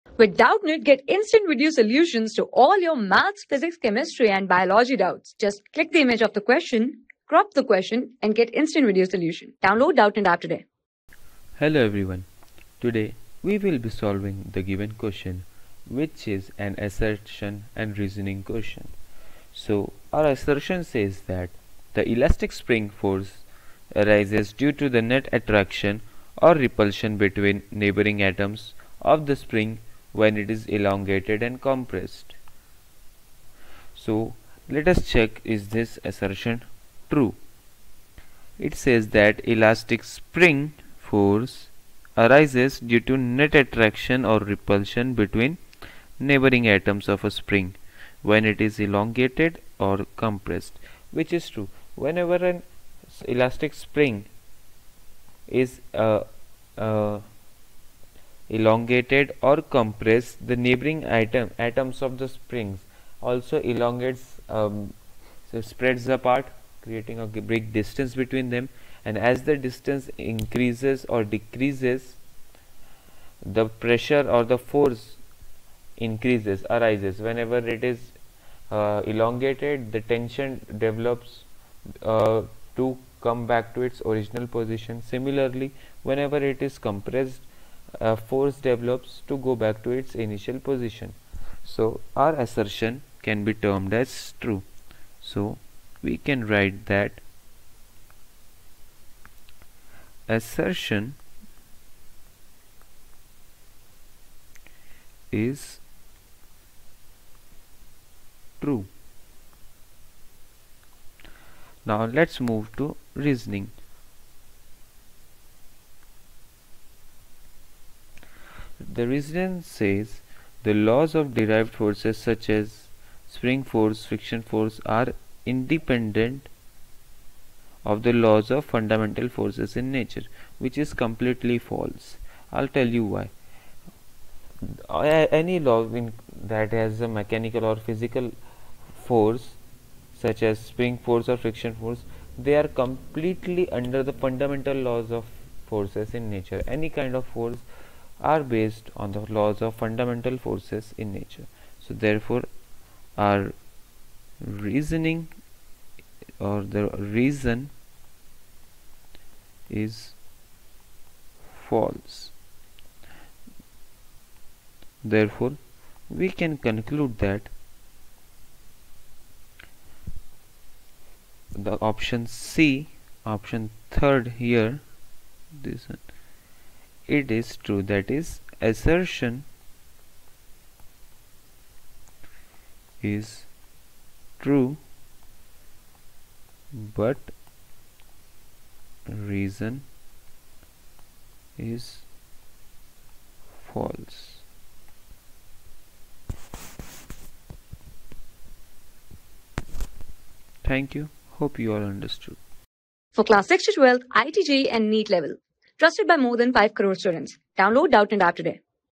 Assertion : The elastic spring force arises due to the net attraction or repulsion between the n...
Assertion : The elastic spring force arises due to the net attraction or repulsion between the neighbouring atoms of the spring when it is elongated of compressed. Reason : The laws of derived forces such as spring force, friction force are independent of the laws of fundamental forces in nature.
Class: 11
Subject: PHYSICS
Chapter: PHYSICAL WORLD
Board:NEET

👉 Have Any Query? Ask Us.